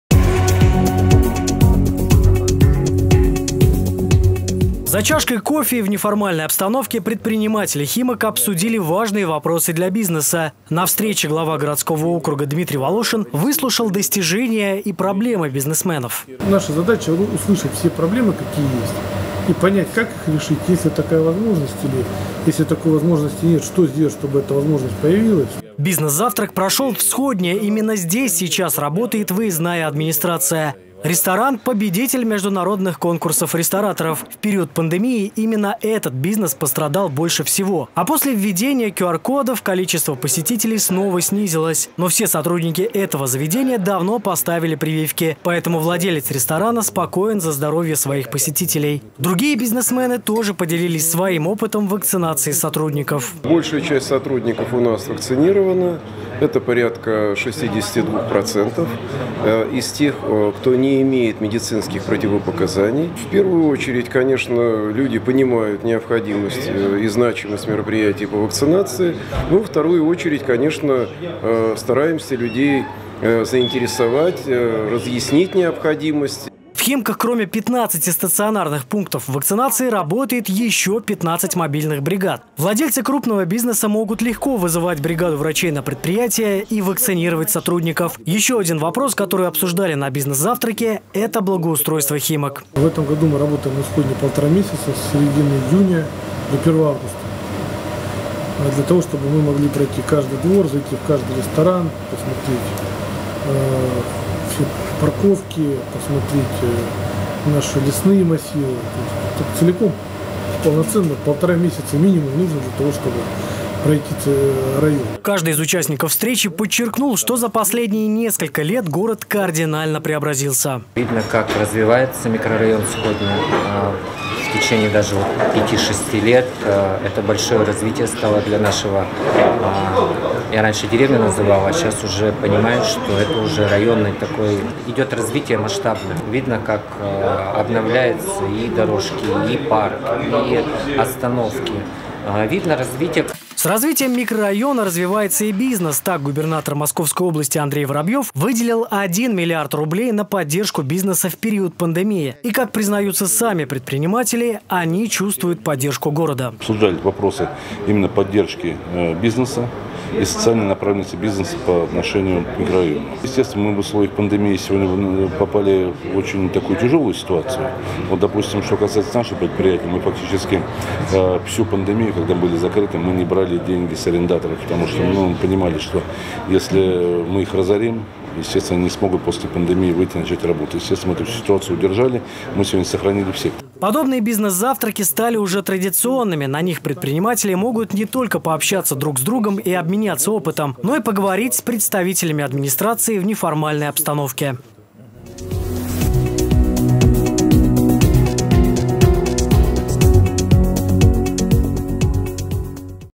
Предприниматели Химок обсудили стратегии развития округа. 15.07.2021
За чашкой кофе и в неформальной обстановке предприниматели Химок обсудили важные вопросы для бизнеса. На встрече глава городского округа Дмитрий Волошин выслушал достижения и проблемы бизнесменов.